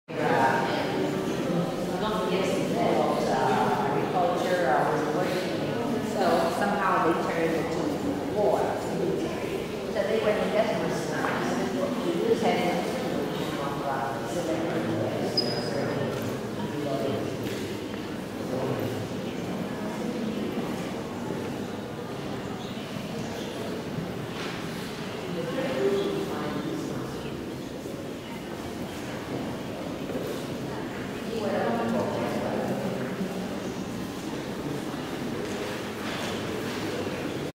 Vatican - Raphael's Rooms- Room of Heliodorus (1512-1514)
Raphael pictures:
Mass of Bolsena;
Liberation of St Peter;
Encounter of Leo the Great with Attila;
Expulsion of Heliodorus from the temple;
Ceiling;